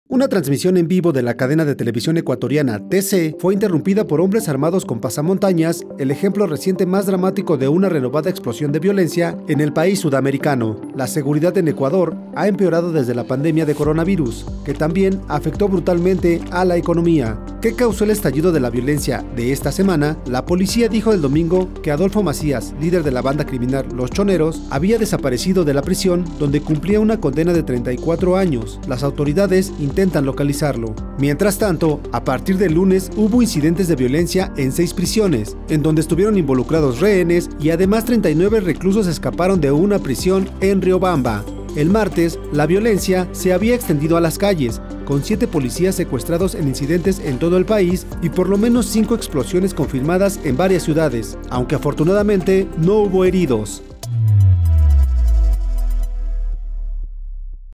¿Qué está pasando en Ecuador?
El nuevo presidente de Ecuador, Daniel Noboa, prometió responder al creciente crimen que obstaculizó a su predecesor, pero enfrenta una batalla cuesta arriba.